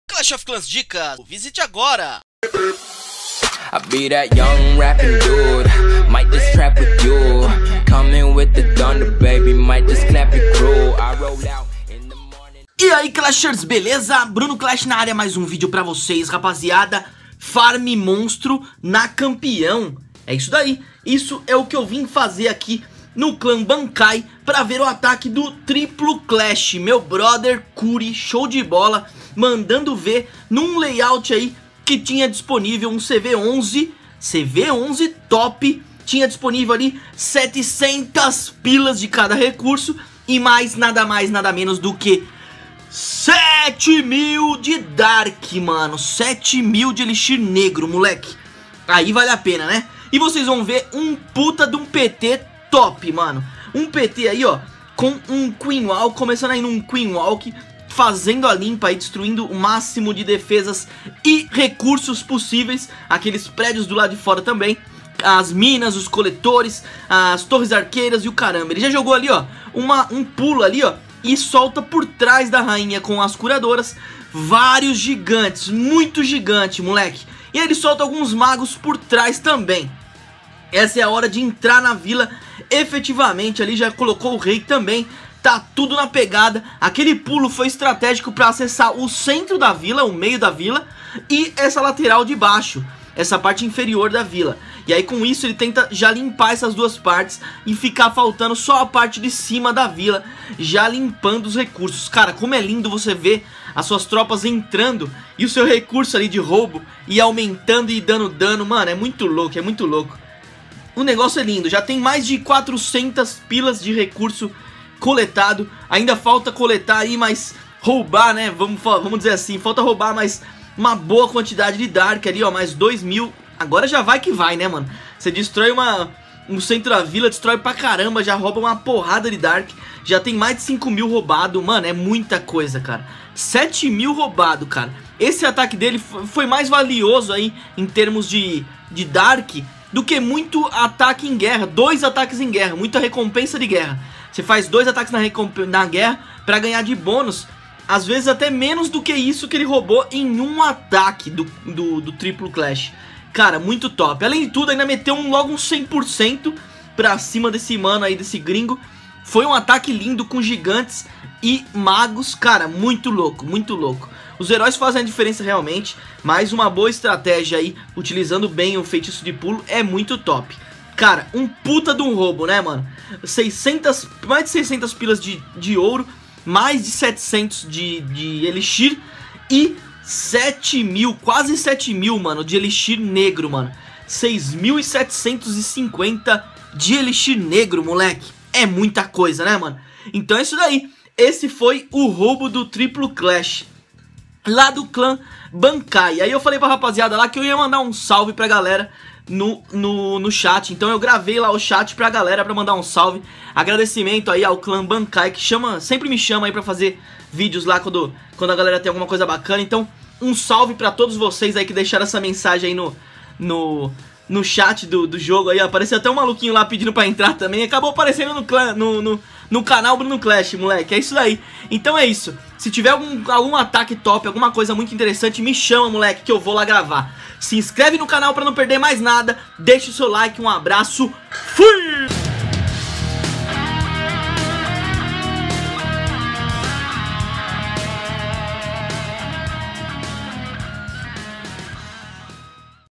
ESPETACULAR ROUBO DE 7 MIL DE ELIXIR NEGRO (DARK ELIXIR) E MILHÕES DE RECURSOS
O ESPETACULAR ROUBO DE 7 MIL DE ELIXIR NEGRO (DARK ELIXIR) E MILHÕES EM RECURSOS!! Um verdadeiro FARM MILIONÁRIO!!

“Go Clash”